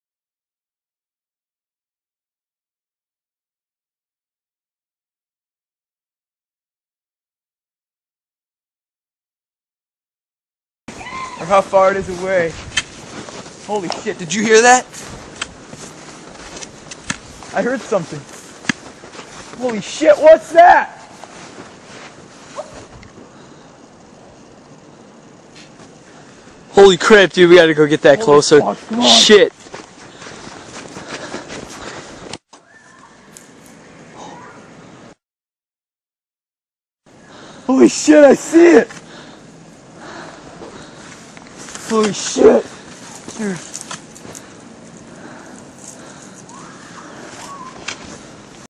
Bigfoot caught on tape! Is it really a sasquatch...you decide!!!
This bigfoot was found in the wilderness of Michigan